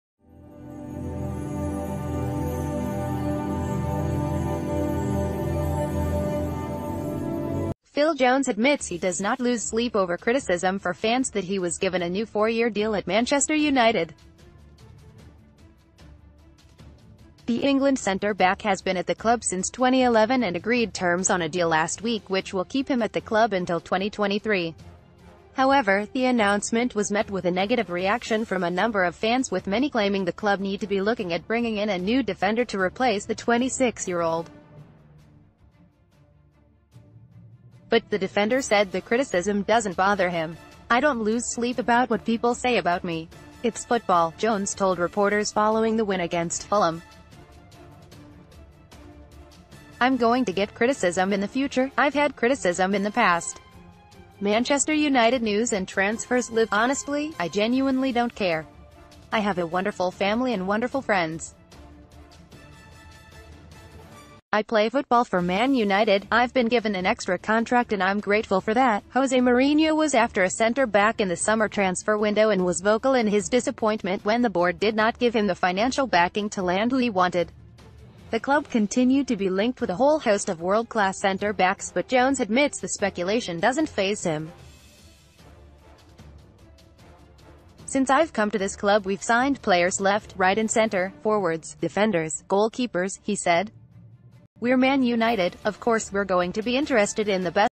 ManUtd News - Phil Jones responds to criticism over his new Manchester United contract
Phil Jones admits he does not lose sleep over criticism for fans that he was given a new four year deal at Manchester United. The England centre back has been at the club since 2011 and agreed terms on a deal last week which will keep him at the club until 2023. However, the announcement was met with a negative reaction from a number of fans with many claiming the club need to be looking at bringing in a new defender to replace the 26-year-old. But, the defender said the criticism doesn't bother...